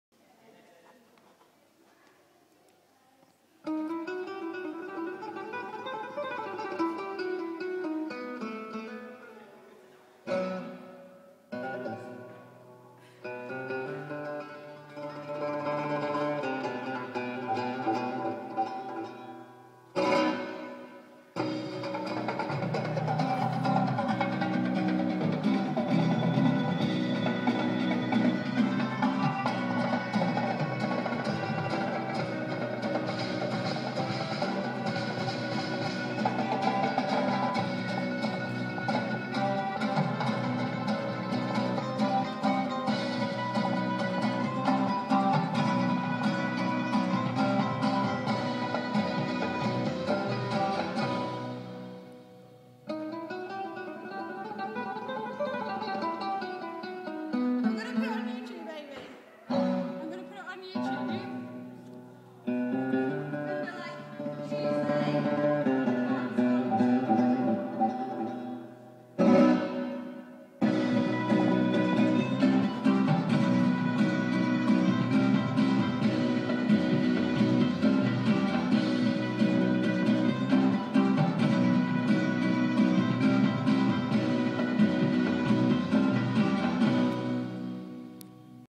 tuesday girls advanced floor routine
my routine for the tuesday girls advanced class. watch and listen for the timings in the music! good luck see you girls next tuesday! x